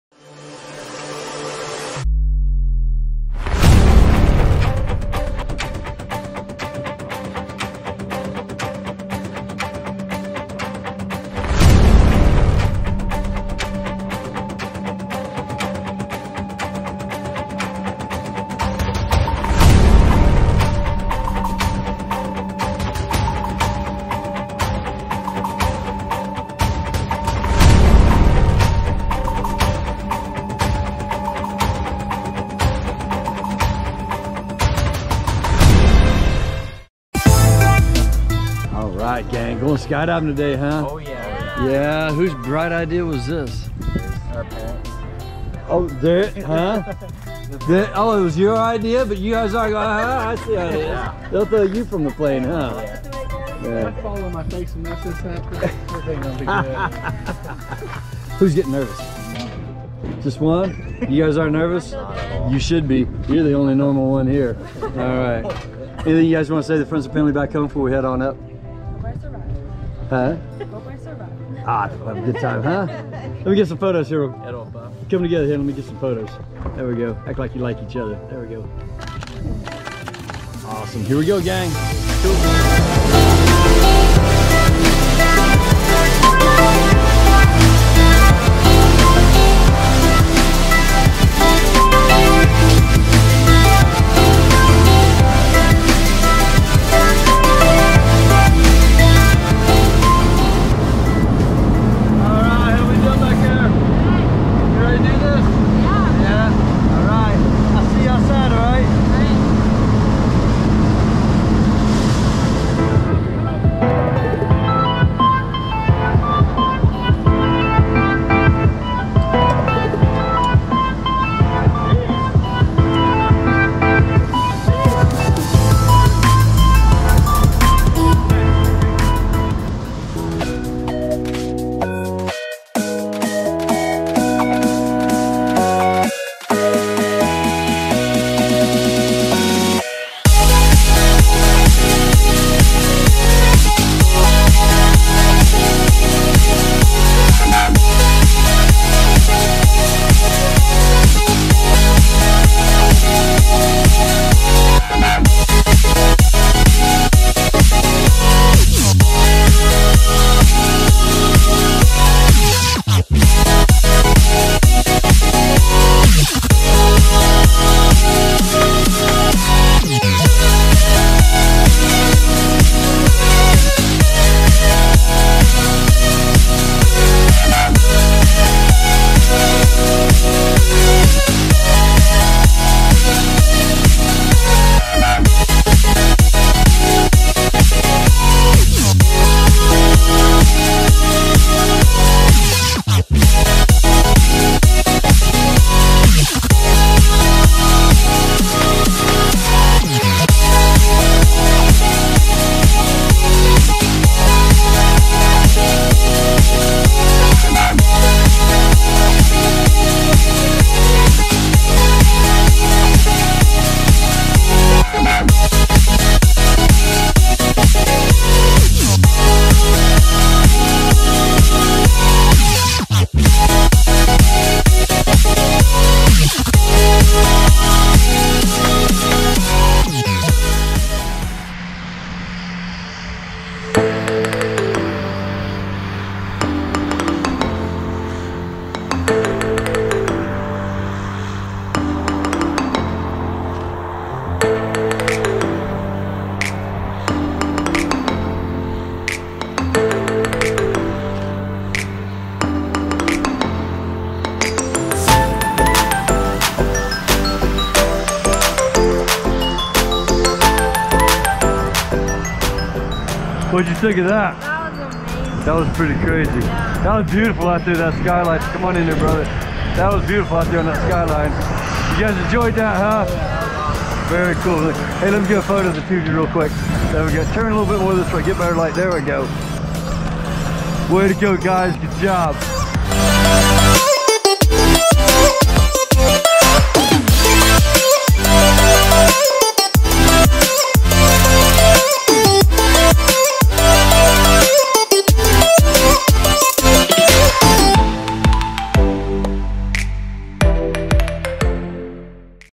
Emma Weger - Tandem Skydive at Skydive Indianapolis
Emma Weger's Tandem Skydive at Skydive Indianapolis!

--- Intro ---

--- Pre Jump ---

--- Freefall Music ---